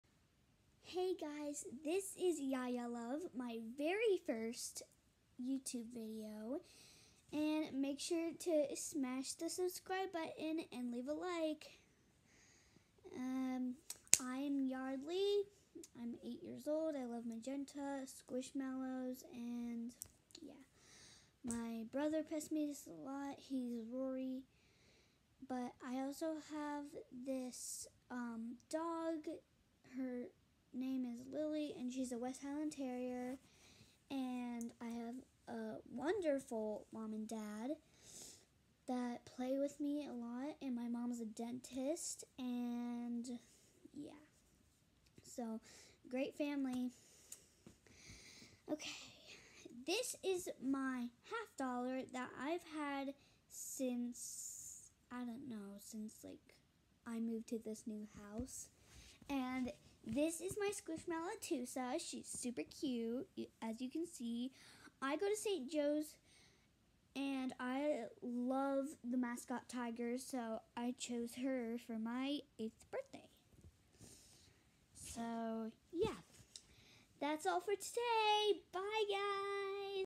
Yardley Introduction
This is Yardley Reichard's first YouTube video.  She introduces herself to the world.  Enjoy!